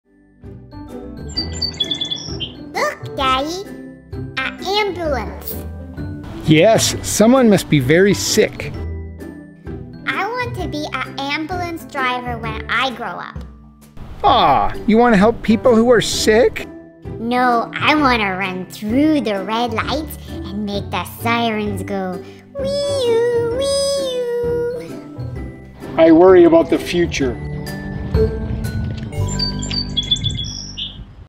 "I like the way it goes Wee Oooo Weee Oooo" 🚑🐶😆 a Biscuit Talky Cricket Chronicles e332
Biscuit REALLY wants to be an ambulance driver when she grows up! 🚑🐶😆. A Biscuit Talky Cricket Chronicles e332

🎥 Follow Us 🎥
All AWESOME Cricket Chronicle Links HERE!